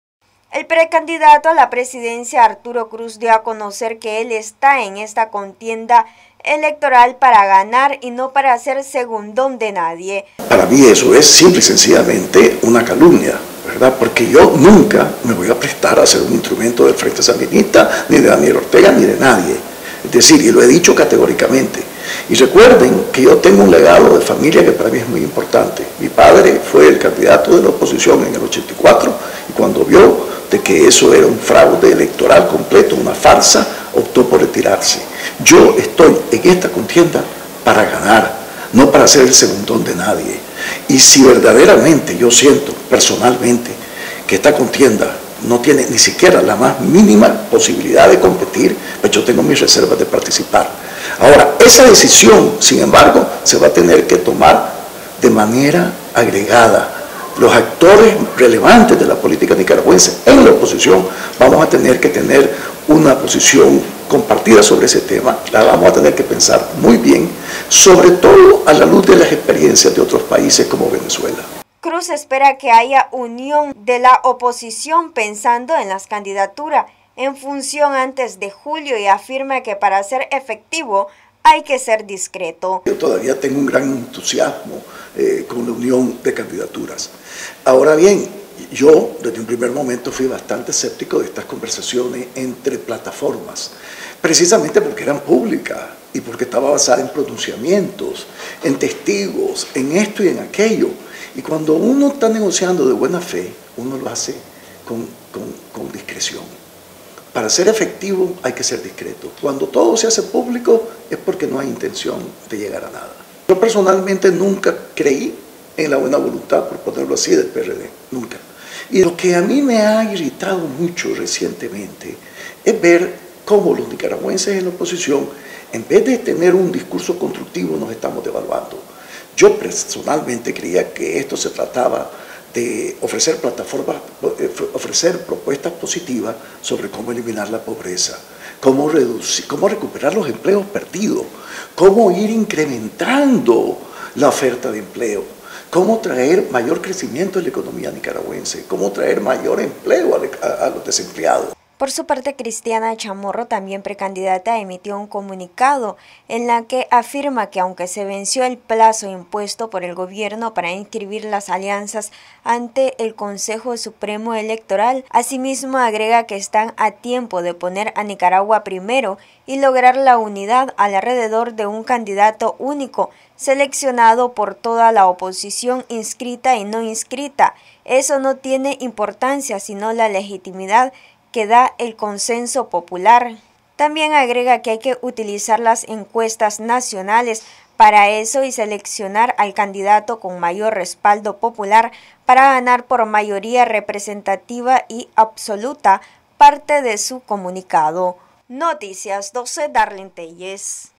Arturo Cruz: Yo nunca me voy a prestar a ser un instrumento del Frente Sandinista
NosResistimosAlEmbargo El precandidato presidencial Arturo Cruz, afirma que está en esta contienda electoral para ganar y no ser segundón de nadie, además agrega que todavía hay esperanza de unidad.